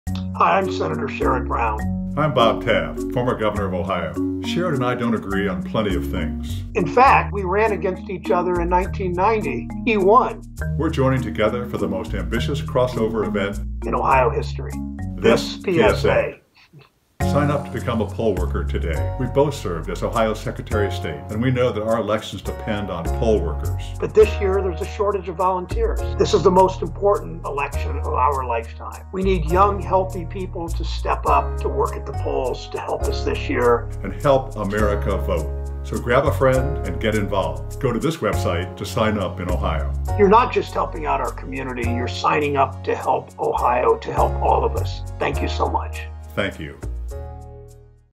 I made this video with the only guy to ever beat me in an election...
Bob Taft, former Governor of Ohio, joins me for this important PSA. Governor Taft unseated me as Ohio's Secretary of State in 1990. But we teamed up on this PSA because we both agree - we need more young, healthy people to sign up to become poll workers.